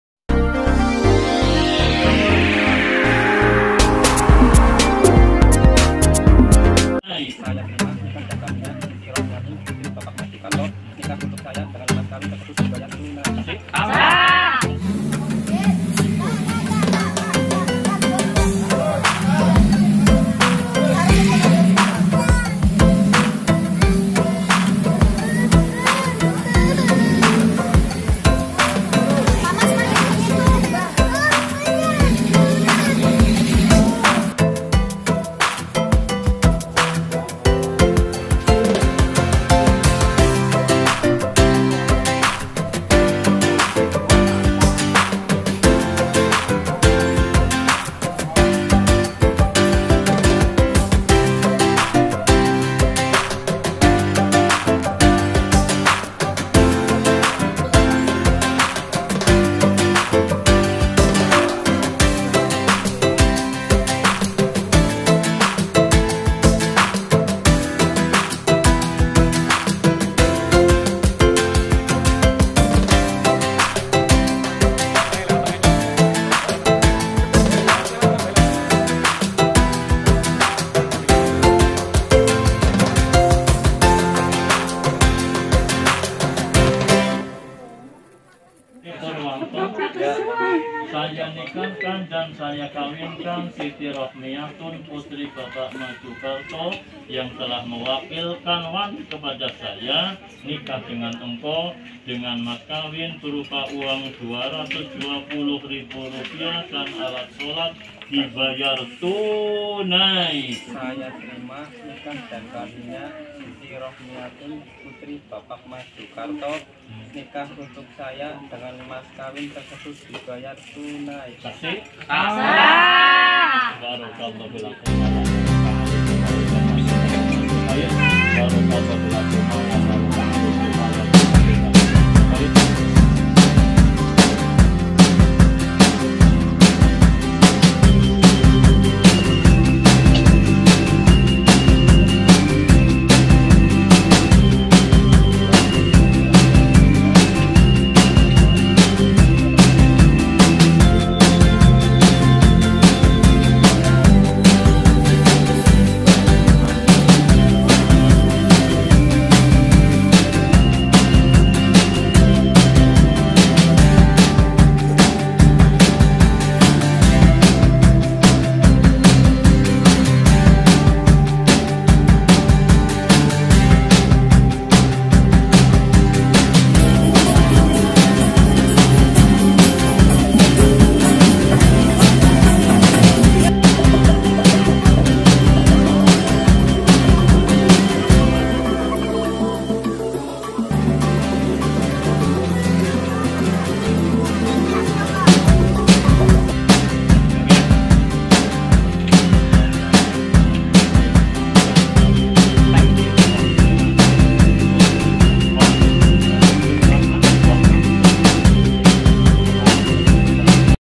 Rewang cermin budaya gotong royong Indonesia Happy wedding Ndoni & Siti
Rewang adalah kebudayaan asli Indonesia. Rewang mengajarkan sikap gotong-royong. Nilai-nilai sosial akan semakin terpupuk melalui rewang. Menariknya tradisi rewang mampu melibatkan banyak orang meskipun berasal dari status ekonomi yang berbeda atau memiliki tingkat pendidikan berbeda pula.